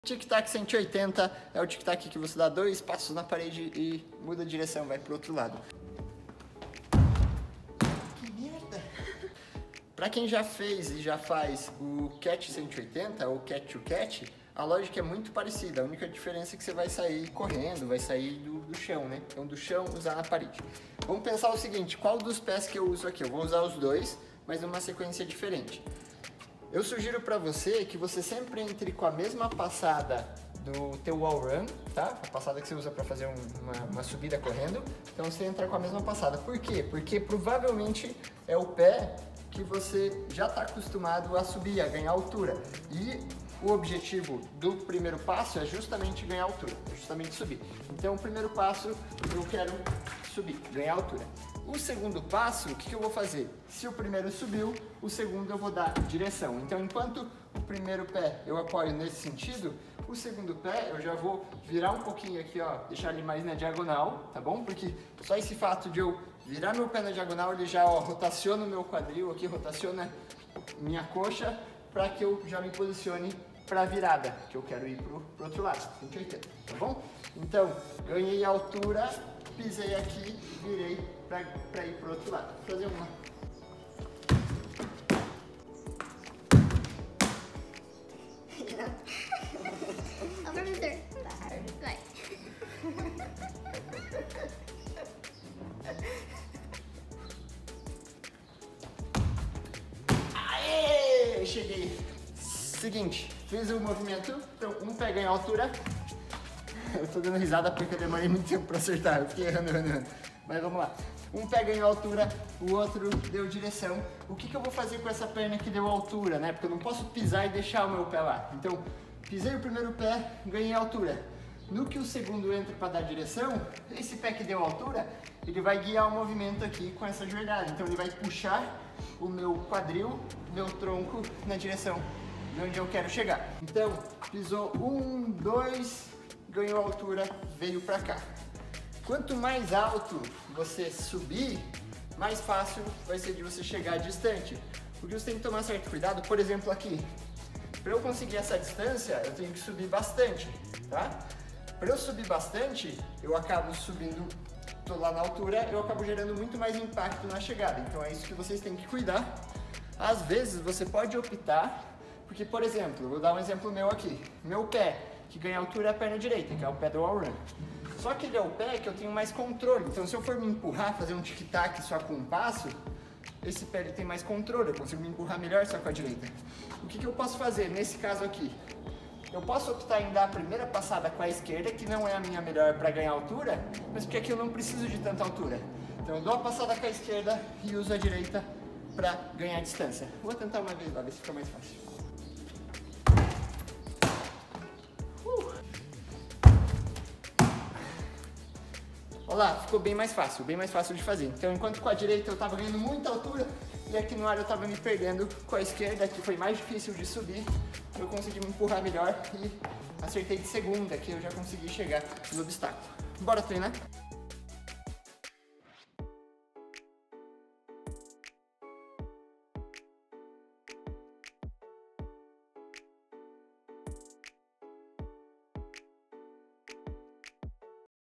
TUTORIAL DE PARKOUR 021 - TIC TAC 180
Tutorias de Parkour. Aprenda a fazer o Tic Tac 180. Curta para continuarmos postando mais vídeos e dicas!